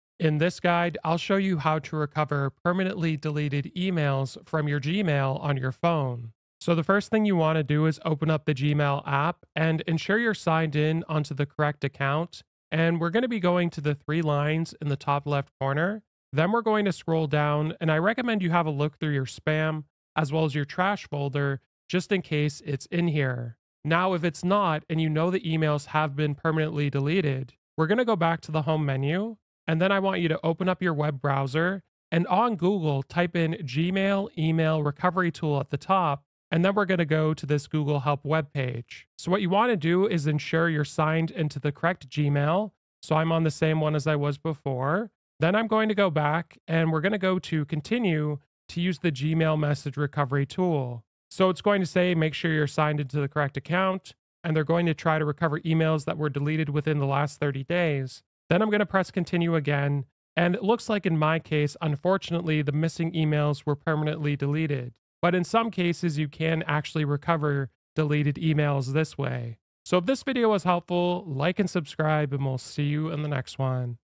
How To Recover Deleted Emails On Gmail (Mobile)
Do you want to know how to recover permanently deleted emails from Gmail on your phone? In this guide, I show you how to recover deleted emails from your Gmail account on your phone. Hope this helps!